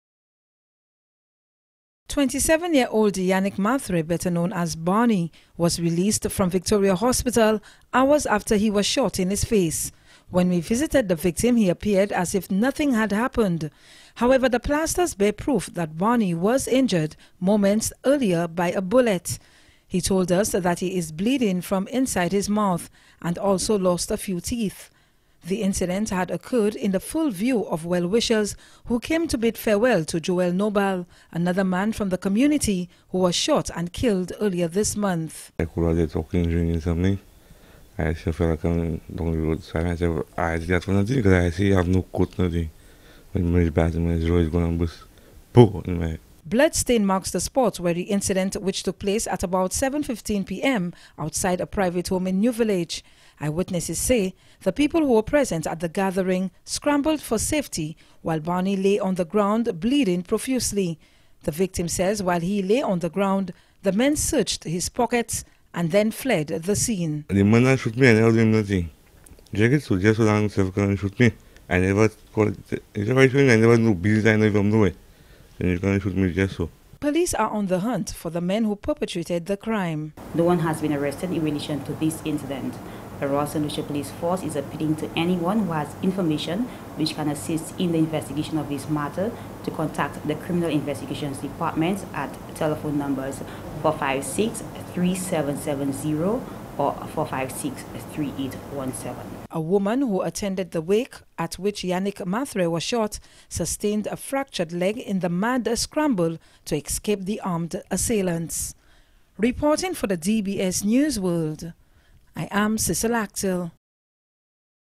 in the line of fire
A man from New Village who was shot in the mouth last night, has been recounting his ordeal.
 Yannick Mathurin was -- ironically, attending a wake for a friend when he came under fire from a lone gunman.
 The bullet grazed his jugular vein, passed through the back of his throat, and exited the side of his mouth, breaking some teeth in the process.
 Mathurin told his story exclusively to DBS's Cecile Actille who begins our news coverage tonight: